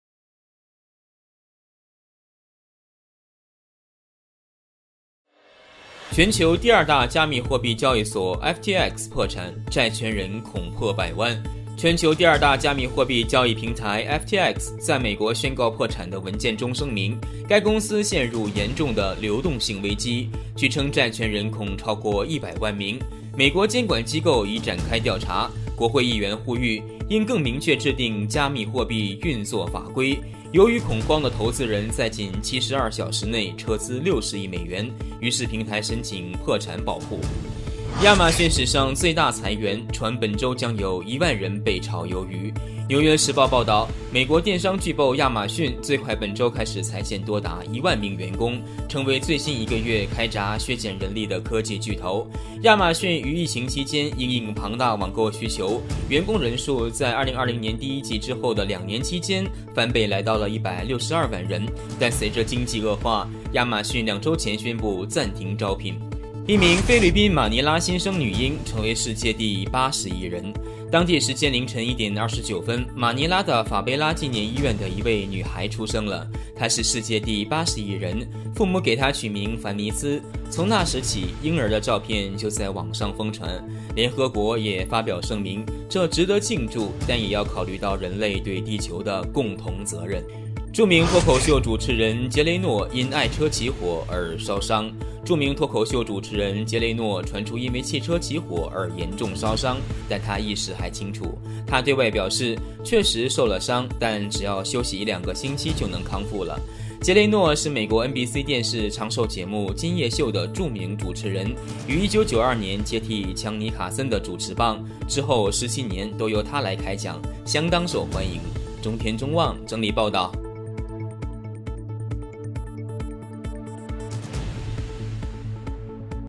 2022.11.15 中天北美新聞-1 北美要聞
新聞的一開始，首先帶您快速瀏覽北美各地的最新要點新聞。

00:00 全球第二大加密貨幣交易所FTX破產 債權人恐破百萬
00:38 亞馬遜史上最大裁員 傳本周將有1萬人被炒魷魚
01:06 一名菲律賓馬尼拉新生女嬰 成為世界第80億人
01:31 著名脫口秀主持人傑雷諾因愛車起火而燒傷